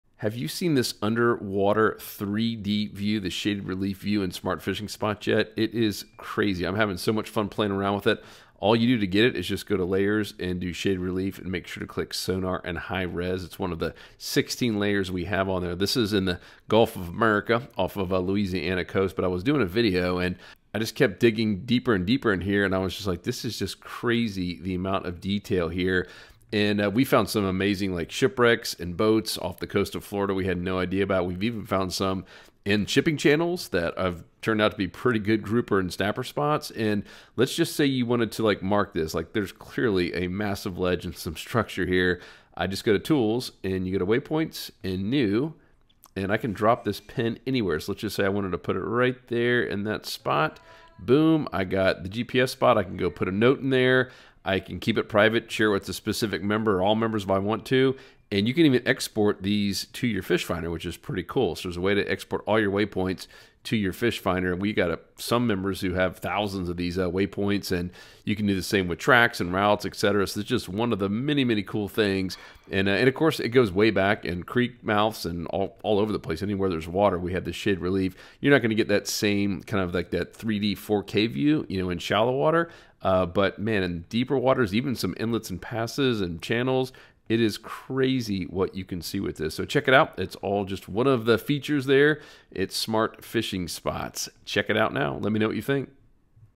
High Res Sonar Imagery Reveals Fishing Hotspots for InshoreFishing and OffshoreFishing
The Smart Fishing Spots Shaded Relief and Sonar imagery helps you find underwater structure that you would never know was there! This imagery allows you to see under the surface to see depth changes and underwater topography.